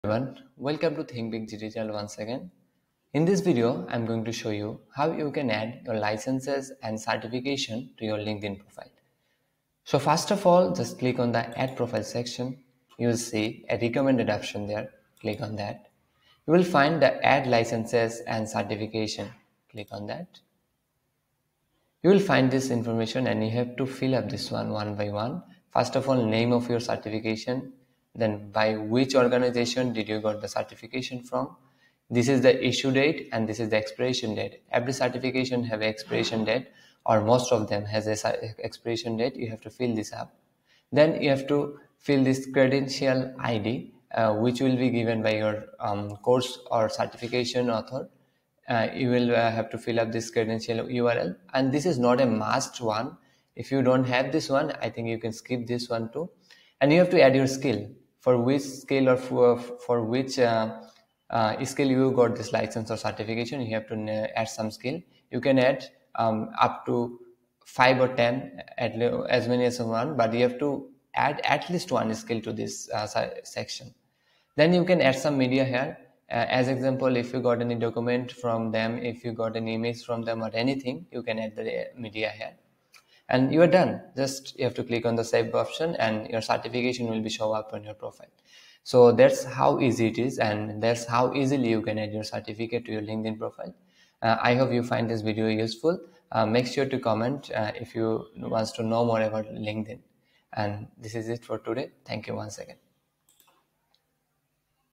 How To Add Licenses and Certifications to Your LinkedIn Profile
In this video, I will show you how to add your licenses and certifications to your LinkedIn profile. I will guide you through the process step by step, including filling out the necessary information, adding skills, and even including media. By the end of the video, you will know exactly how to showcase your certifications on your LinkedIn profile.